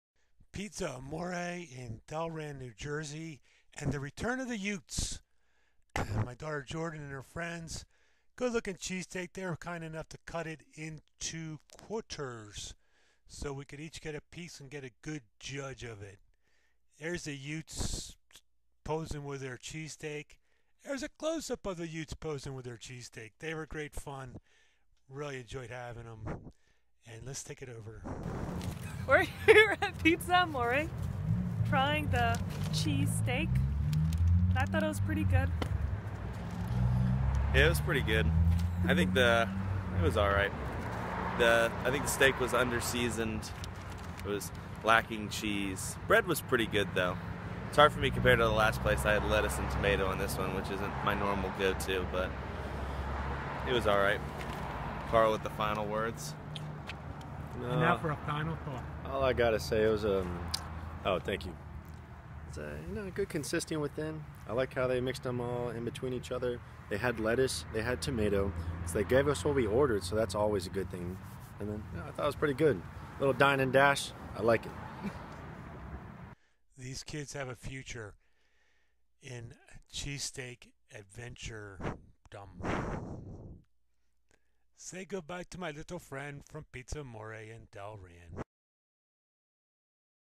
Is the Best Philadelphia Cheesesteak at Pizza Amore?
Philadelphia Cheesesteak Adventure at Pizza Amore, 77 Hartford Rd, Delran NJ.  Cheesesteak scored 88/100.

Welcoming back 'The Youths.'  Daughter Jordan and her friends.  One of them is her husband now.

I went to Pizza Amore with high hopes, that may have been a mistake.  It was nice, they cut the Cheesesteak into quarters for us and we got a free calendar.  The pizzas looked amazing but the Cheesesteak was slightly above average.